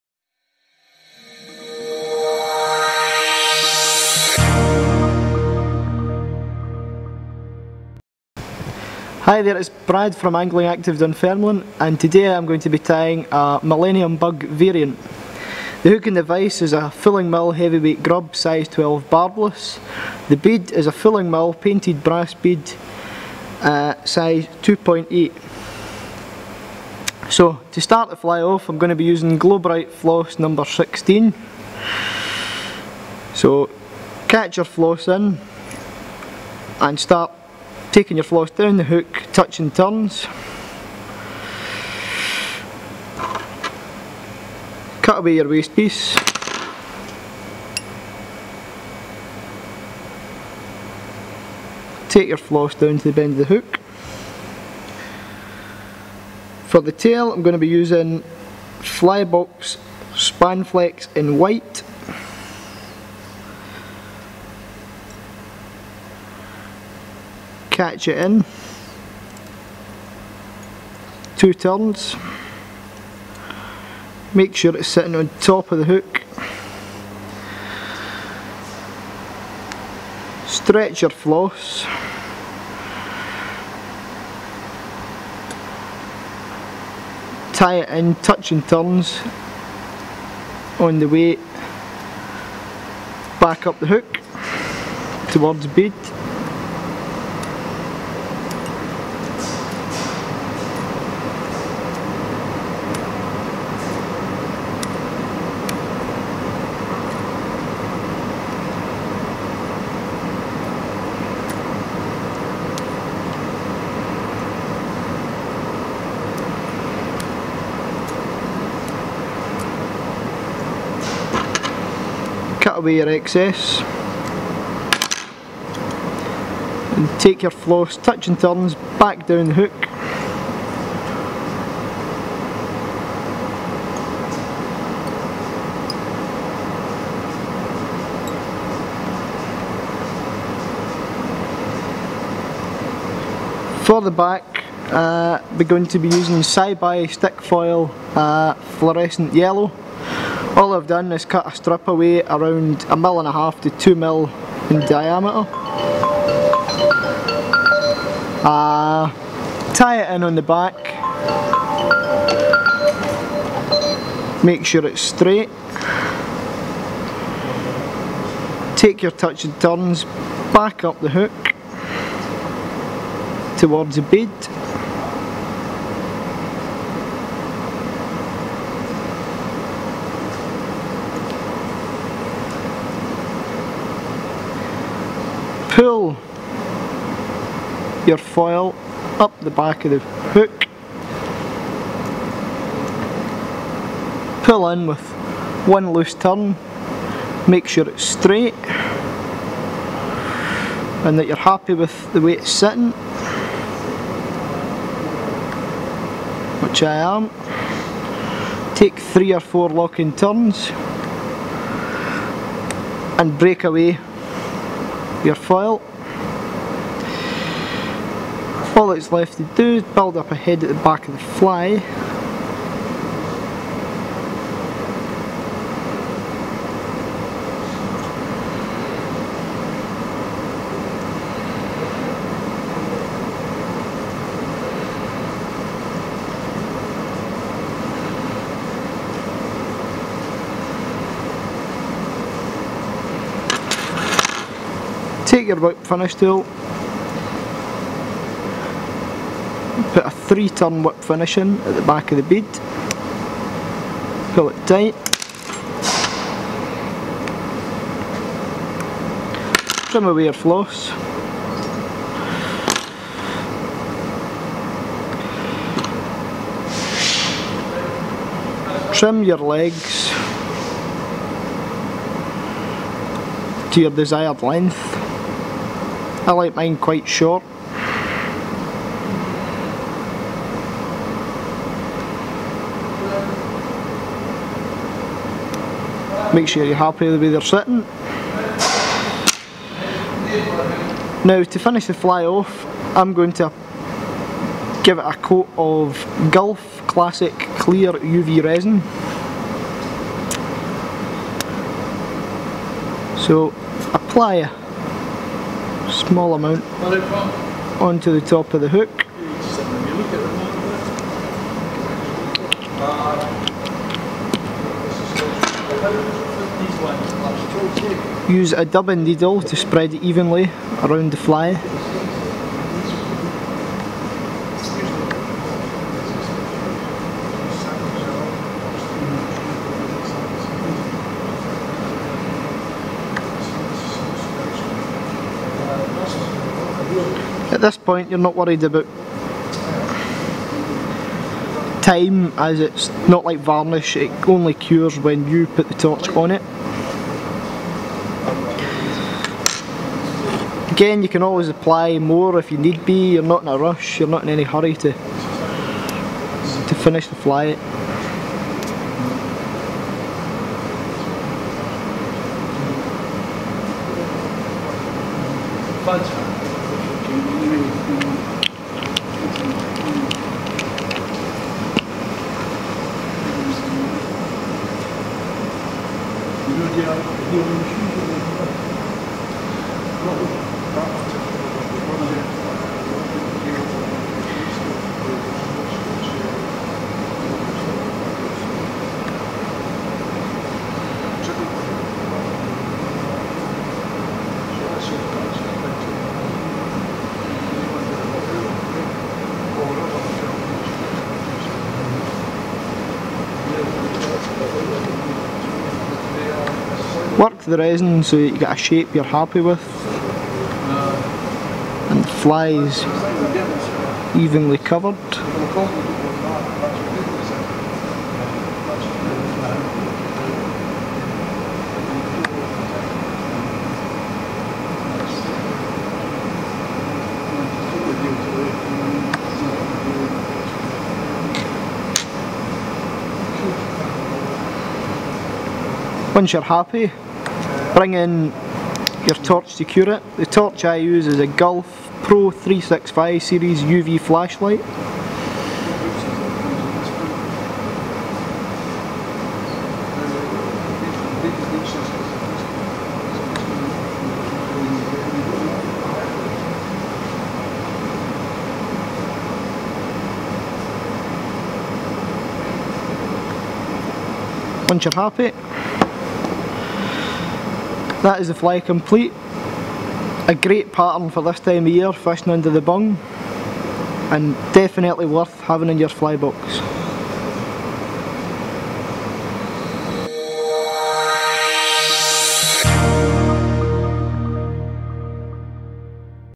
Tying a Millennium Bug Variant - Effective Winter Trout Pattern
Former Scottish Youth International and Angling Active Dunfermline staff member, Brad Gair shows us how to tie a very simple yet effective Millenium Bug variant. An ideal pattern for targeting winter Stillwater trout.